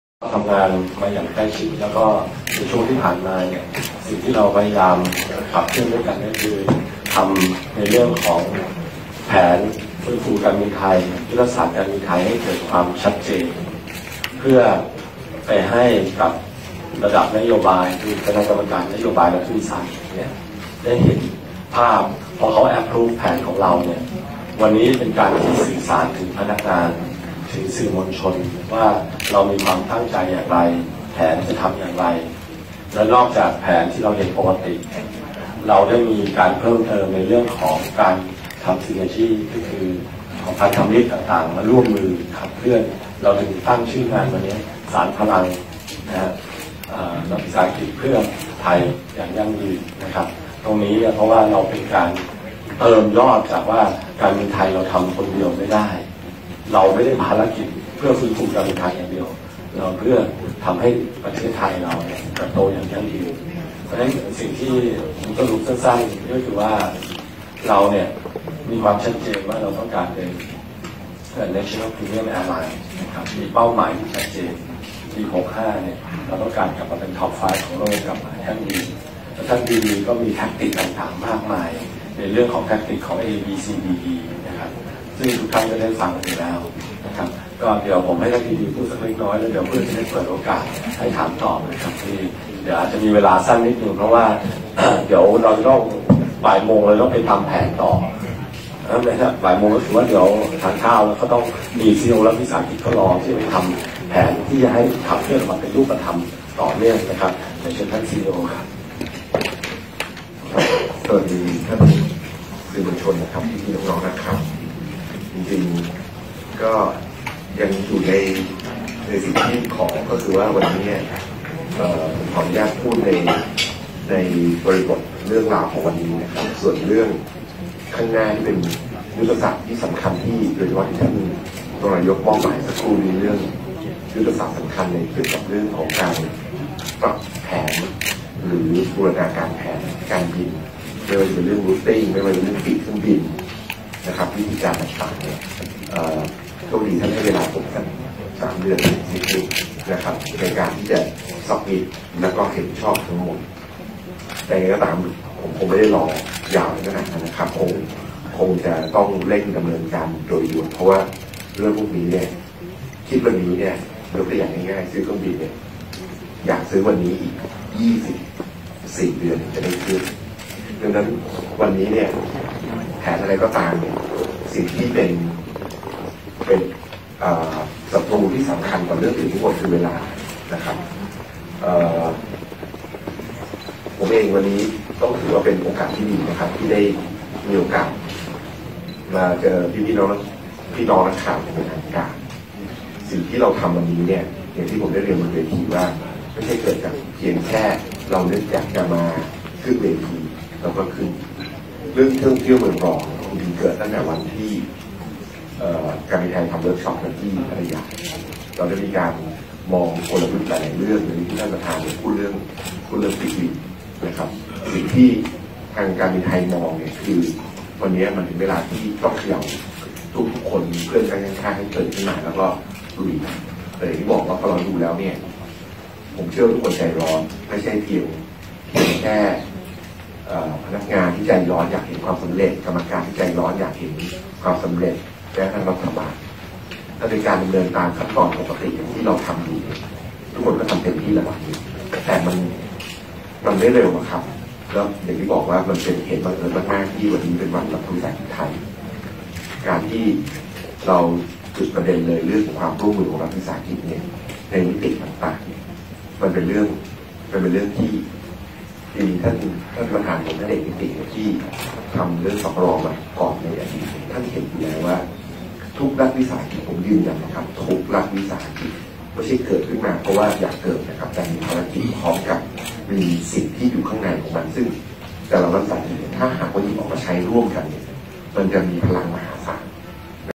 การบินไทยรื้อแผนซื้อฝูงบิน 23 ลำ “สมคิด” ขีดเส้น 3 เดือน - “สุเมธ” ลั่นปี 63 หลุดพ้น-ปี 65 ไม่ขาดทุน
20/09/2018 การบินไทยรื้อแผนซื้อฝูงบิน 23 ลำ “สมคิด” ขีดเส้น 3 เดือน - “สุเมธ” ลั่นปี 63 หลุดพ้น-ปี 65 ไม่ขาดทุน
“สมคิด” กระตุ้นการบินไทยเตรียมจัดยุทธศาสตร์ ผนึก 3 รสก.วางเครือข่าย ขับเคลื่อนธุรกิจเดินหน้าแผนฟื้นฟู เป้าติด Top 5 ของโลกภายในปี 65 ขีดเส้น 3 เดือนคำตอบต้องชัด “เอกนิติ-สุเมธ” ยันรื้อแผนซื้อฝูงบิน 23 ลำ แสนล้านใหม่ มุ่งเพิ่มรายได้ และกำไรที่เข้มแข็ง ปี 63 หลุดพ้นปัญหา ปี 65 ต้องไม่ขาดทุน